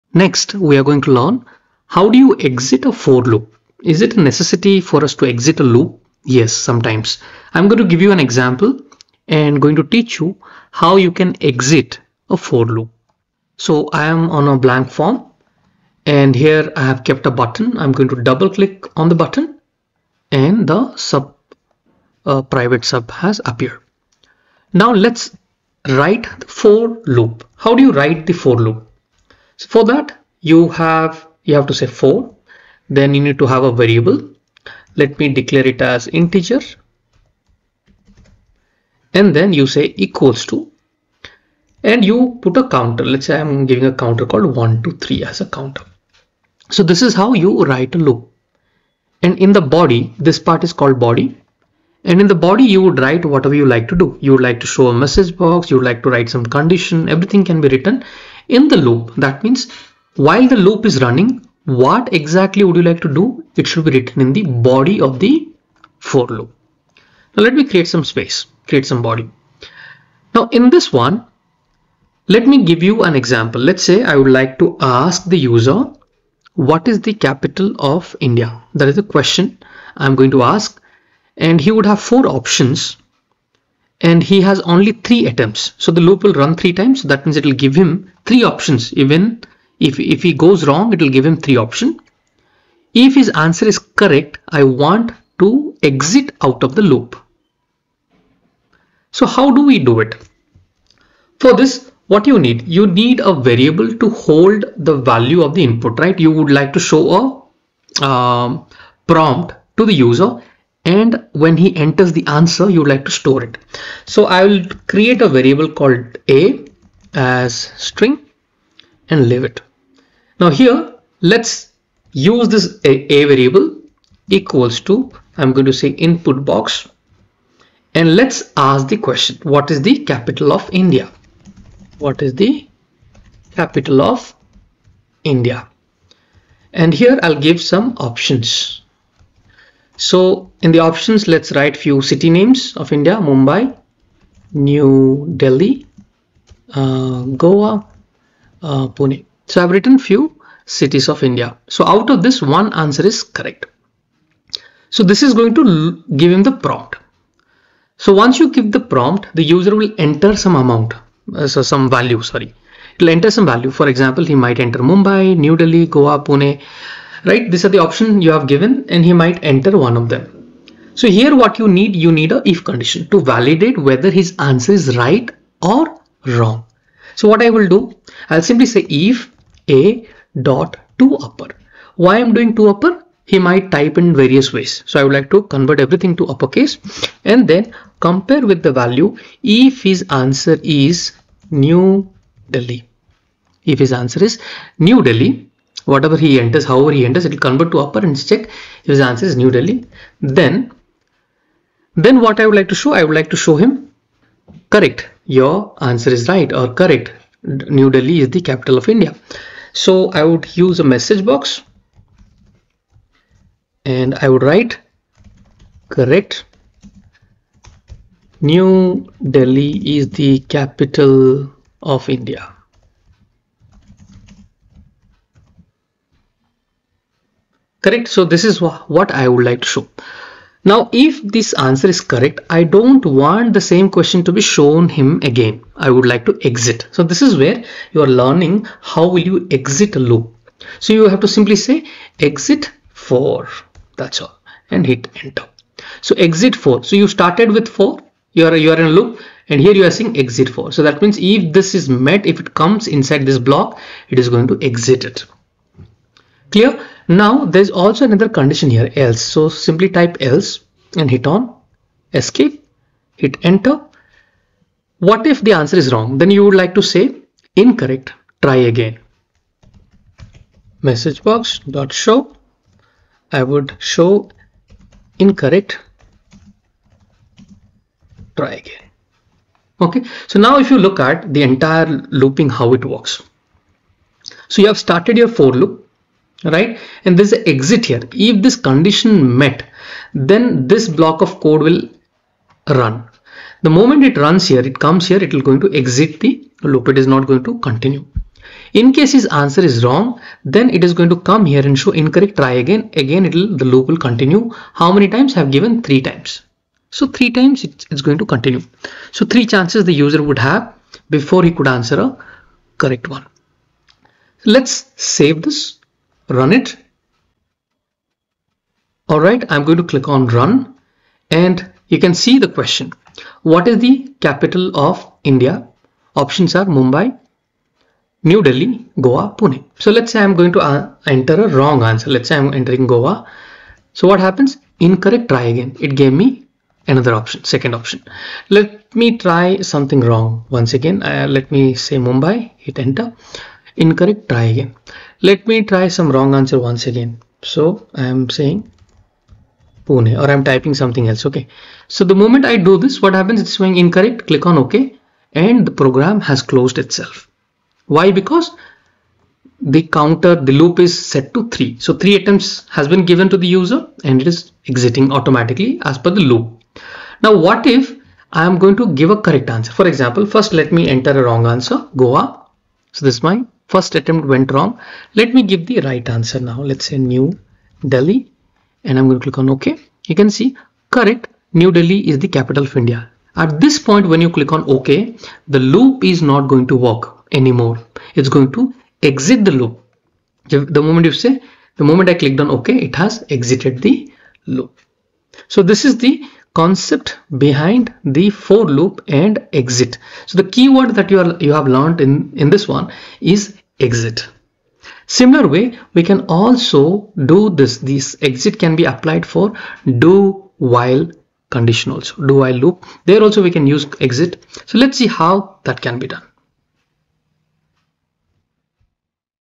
Exit a For Loop in Visual Basic | VB.Net Exit For Loop
In this video, you will learn how to use the Exit For statement in VB.NET to immediately exit a For loop when a certain condition is met. We’ll go through examples where Exit For helps optimize code by stopping the loop early, making it efficient for cases where continuing the loop is unnecessary. Perfect for beginners learning to control loop flow in VB.NET.